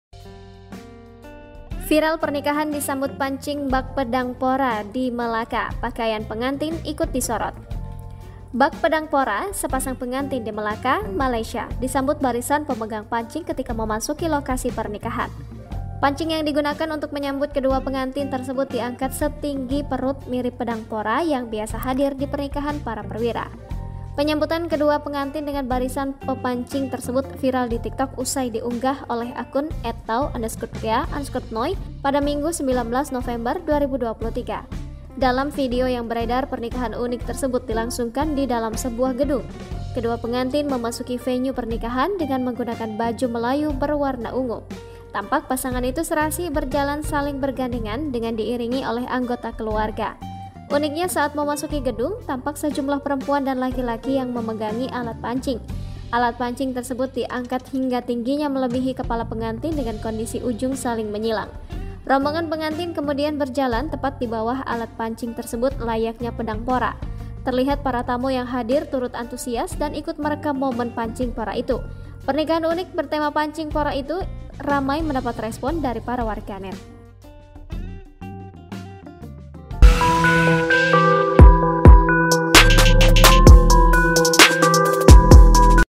Viral Pernikahan Disambut Pancing Bak Pedang Pora di Melaka , Pakaian Pengantin Ikut Disorot
Bak Pedang Pora , sepasang Pengantin di Melaka , Malaysia disambut barisan pemegang pancing ketika memasuki lokasi pernikahan.

Pancing yang digunakan untuk menyambut kedua pengantin tersebut diangkat setinggi perut mirip Pedang Pora yang biasa hadir di pernikahan para perwira.

Penyambutan kedua pengantin dengan barisan pemancing tersebut Viral di TikTok usai diunggah oleh akun @tao_kea_noi , Minggu ( 19/11/2023 ).

Zonaberitanews/safwan